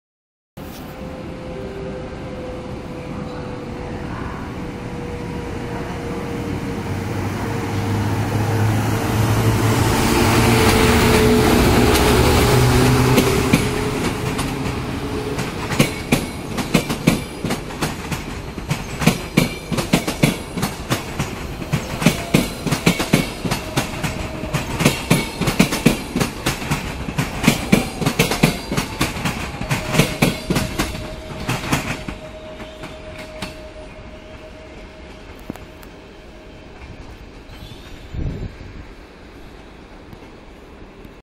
EP09-037 z EIC 6109 Górnik Wrocław Głowny - Warszawa Wschodnia
Wyjazd EP09-037 EIC Górnik  6109 z Wrocławia Głównego do Warszawy Wschodniej zjeżdża na stację postojową Warszawa Grochów.